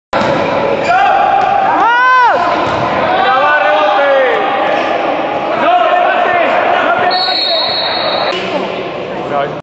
Temporada 2007/2008 Jornada 02: Playground - Pegaso
Los jugadores de Playground sudan la camiseta, y ningún balón se da por perdido. ¡Esa es tuya, Manuel!